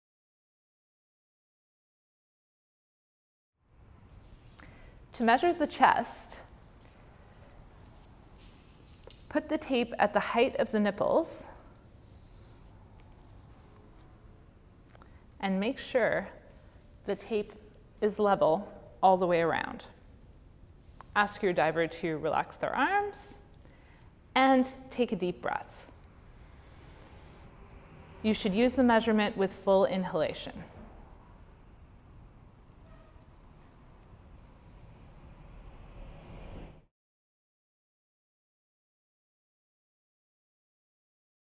Chest Measurement
With both arms at sides, measure circumference at nipple level. Measurement should be taken with breath inhaled. Make sure the tape is level all the way around the chest.